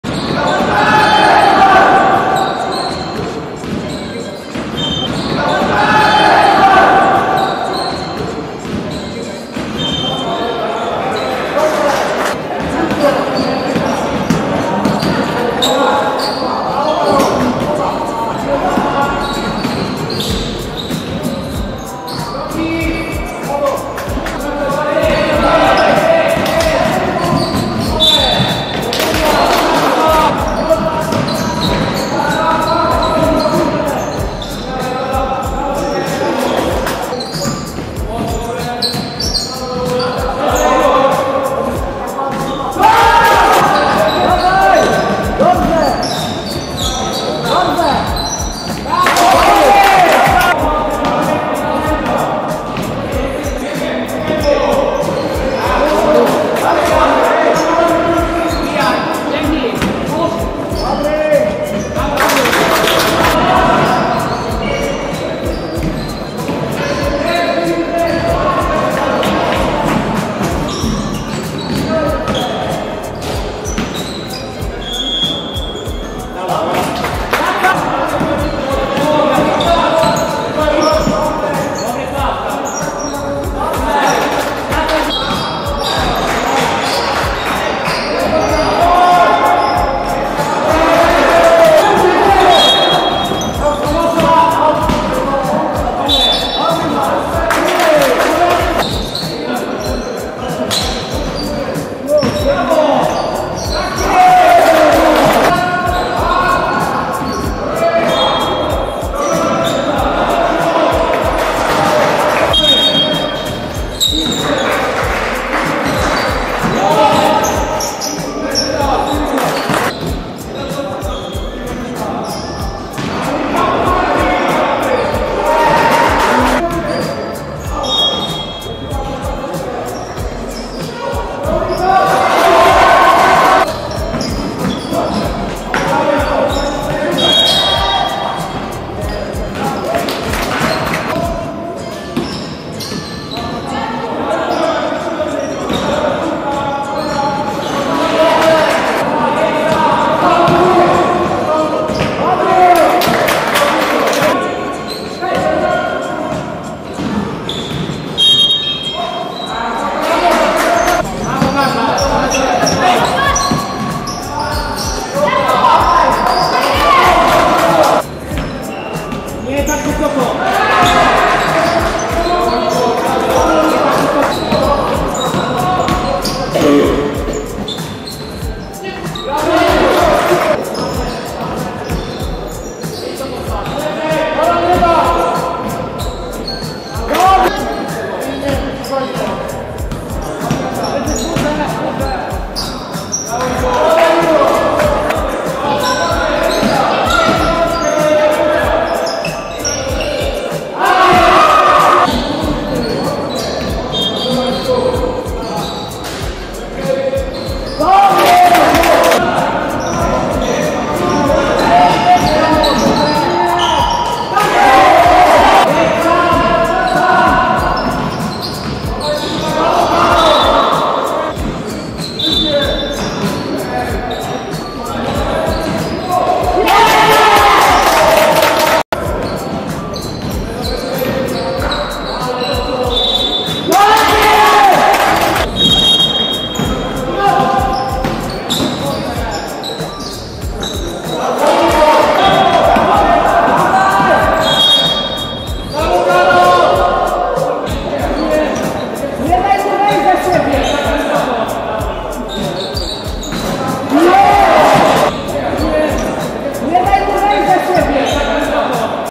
07 12 Półfinał California vs Washington – Liga Amerykańska 18
CALIFORNIA 50:55 WASHINGTON
I.Kudła 17pkt 5zb 5ast
K.Jaworski 16pkt 18zb 3ast